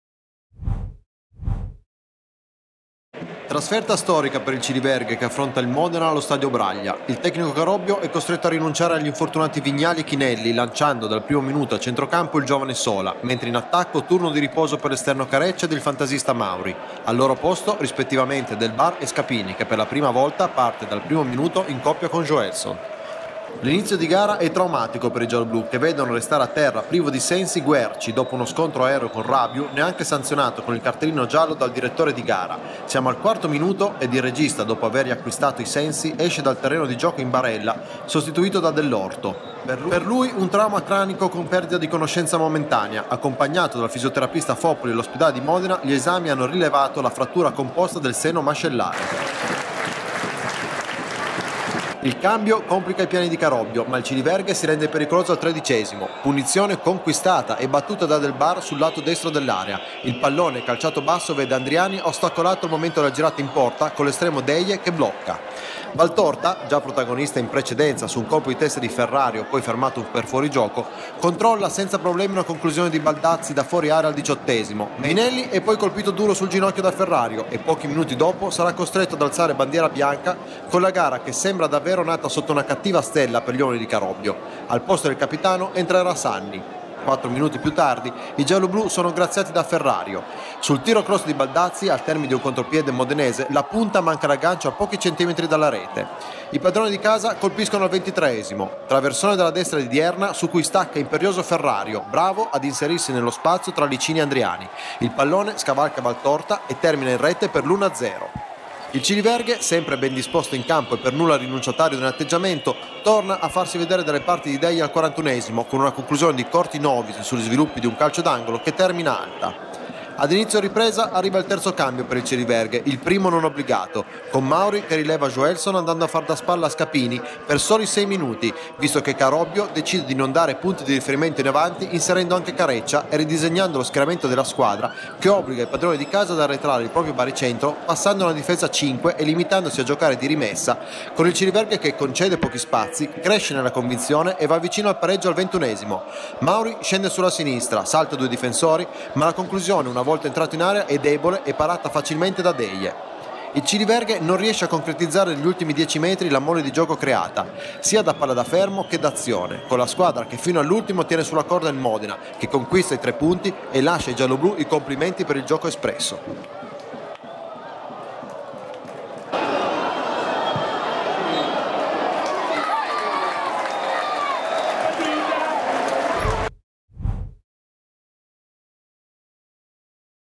Campionato | Modena-Ciliverghe | la sintesi
Le immagini salienti della sfida tra Modena e Ciliverghe terminata 1-0 per i canarini